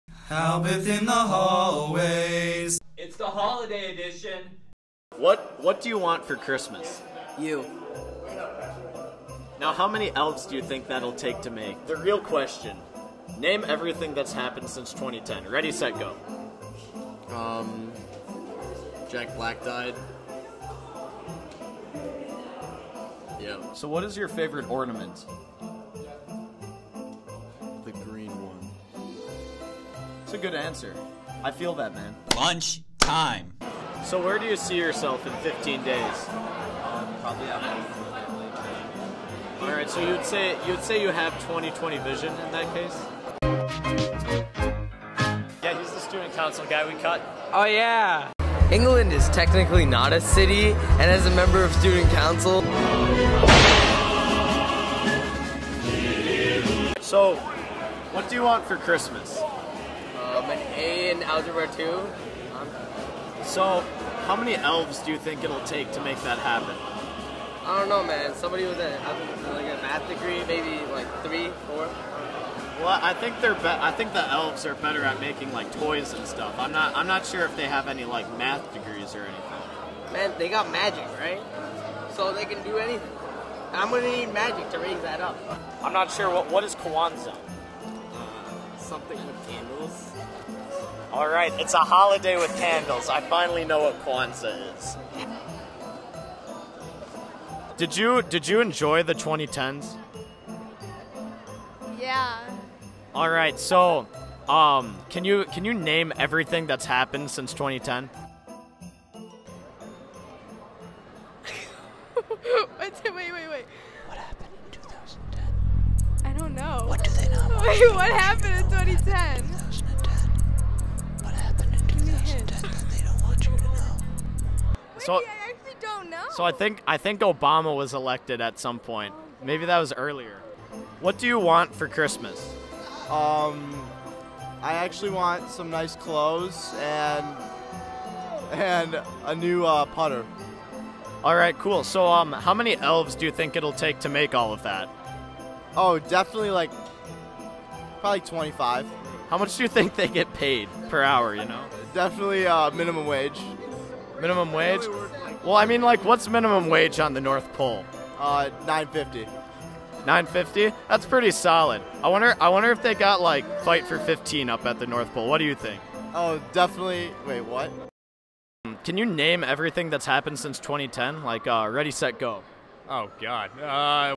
Halboth in the Hallways: Holiday Edition
Created by Jack Wilson and Aaron Halboth at Novi High School for the Cat's Eye News. Aired on December 19, 2020.